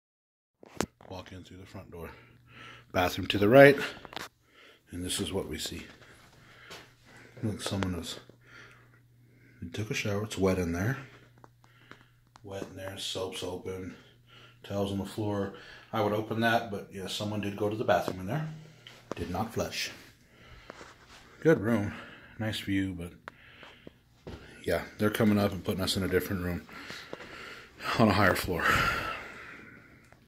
Hilton Los Angeles universal city
They put in someone else room.